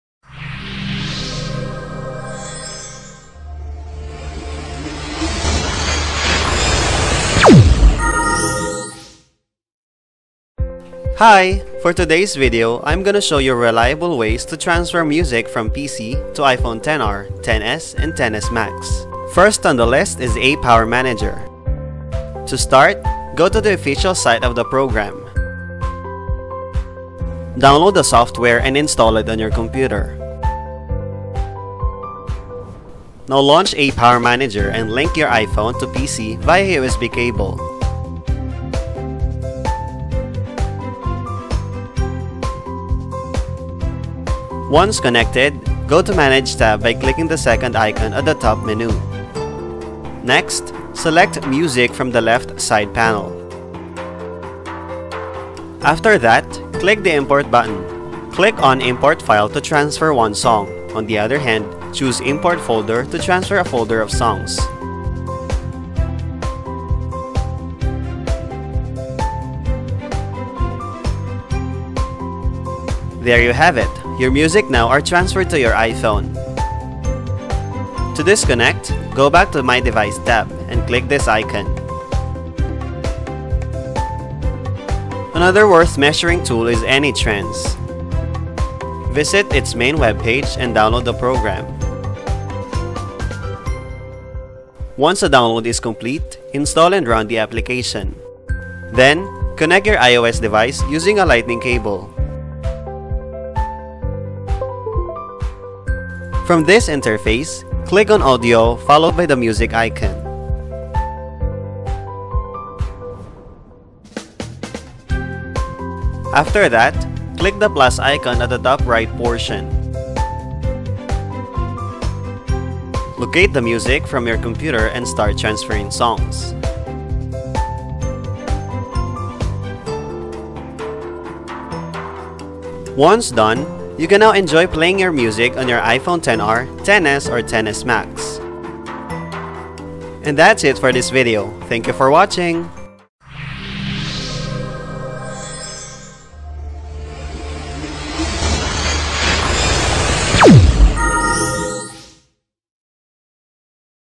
Best Ways to Transfer Music from PC to iPhone XR/XS/XS Max
If you get a new iPhone XR/XS/XS Max, you may need to transfer music from PC to iPhone XR/XS/XS Max and enjoy listening to them on your new iDevice. This video introduces 2 practical ways to do it.